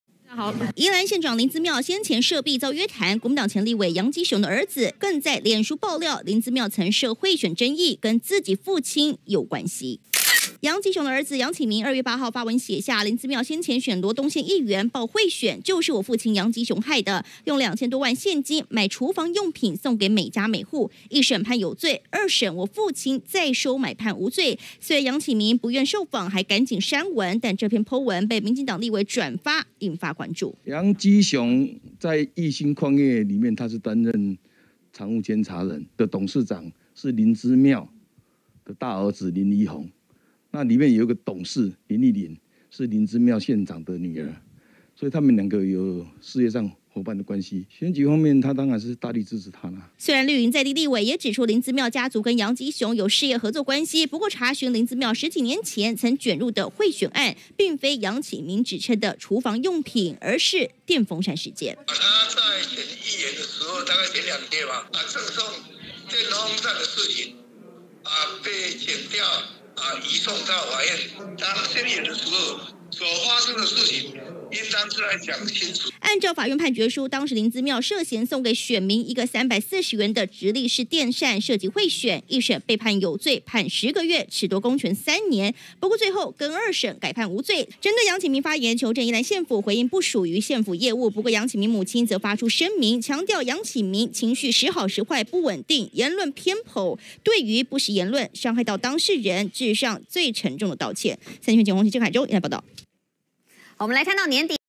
楊吉雄、林姿妙、楊耀火、林騰煌出來說清楚！2022 02 12 08 04 12
「林騰煌是我叔叔楊耀火用百萬現金強迫他的主任秘書潘哲斌教官蘇澳一家ㄧ家去買票！」楊棨名爆料，林騰煌為了勝選迫於無耐只能服從，一審宣判有罪定讞，「二審我叔叔楊耀火出現金50萬、林騰煌出現金50萬，二審又宣判無罪。」

文末，楊棨名強調，「台灣加油，民主法治運動的革命是永不止息的！台灣加油。」